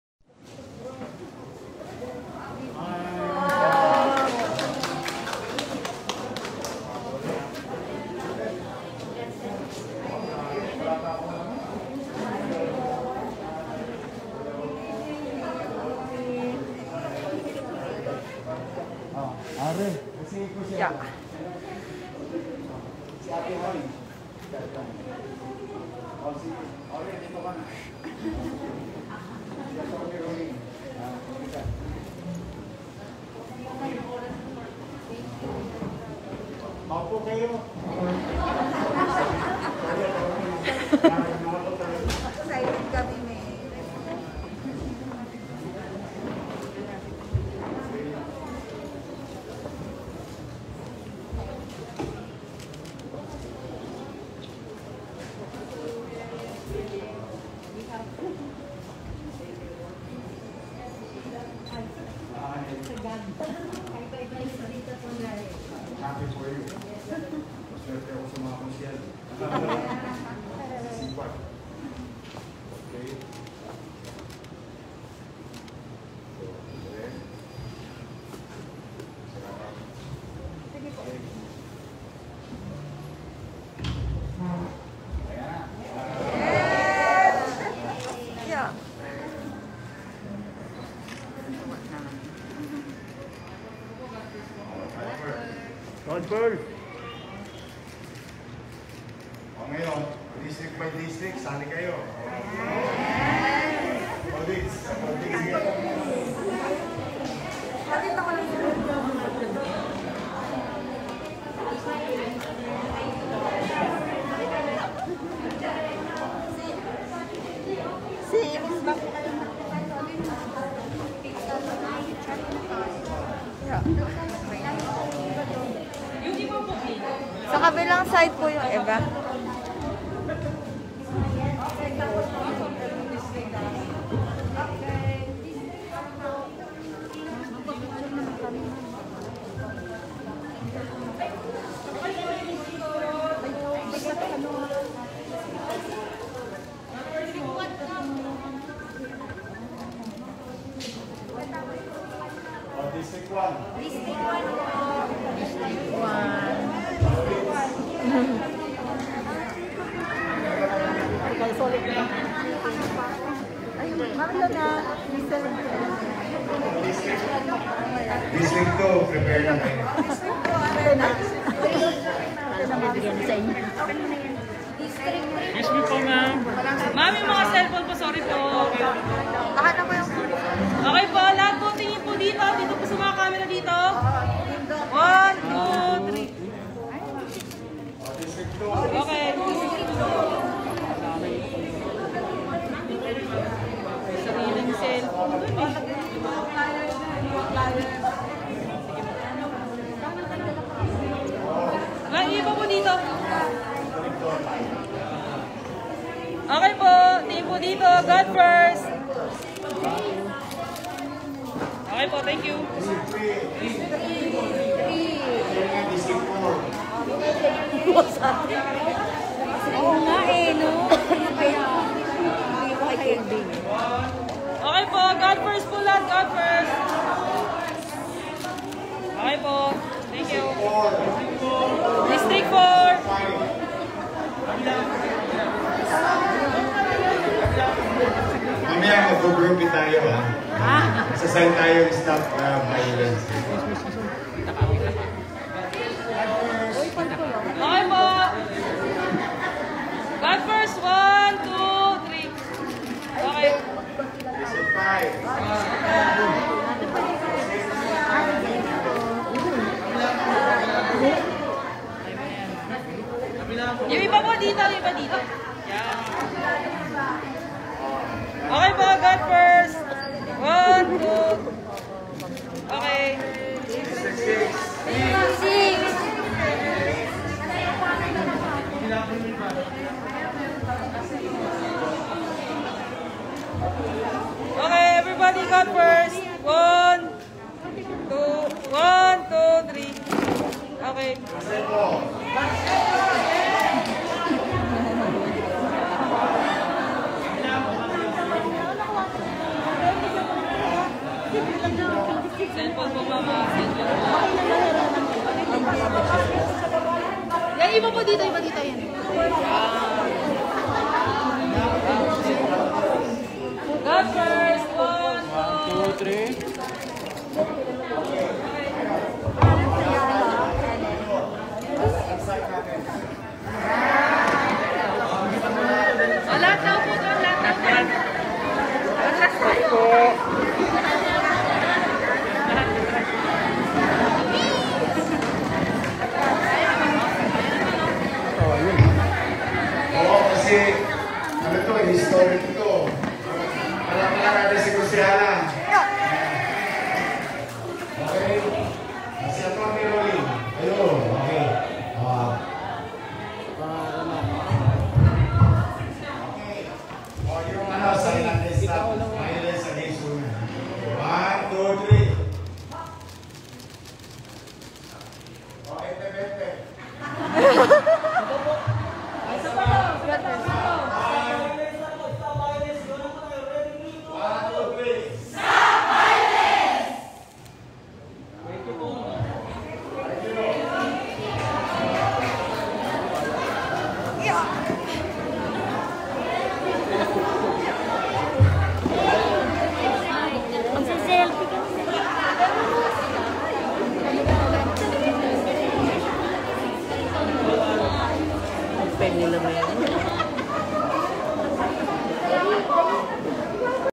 Signing of City Ordinance creating the LCAT-VAWC | Mayor Isko Moreno
Signing of City Ordinance creating the Local Council on Anti-Trafficking and Violence Against Women and their Children (LCAT-VAWC)